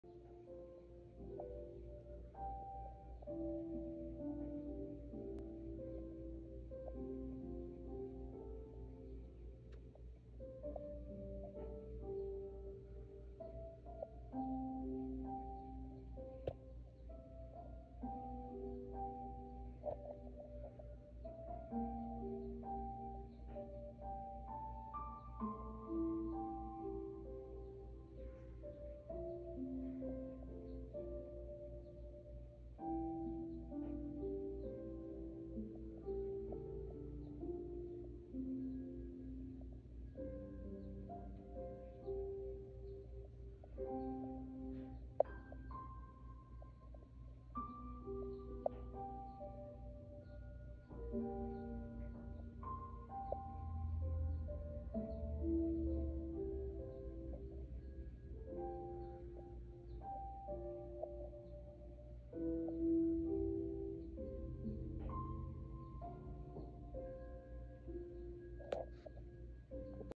Adorable kitten making biscuits
Lady Razzie the Bengal loves to make biscuits with her adorable toe beans